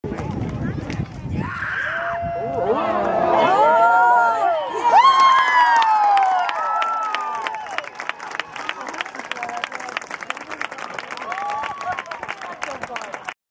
Summer Track Meet - Championship Softball Throw
A super motivated 13 - 14 girl throws a softball an amazing 183 feet 2 inches during the Hershey Qualifier Summer Track Meet at Essex High School, Vermont.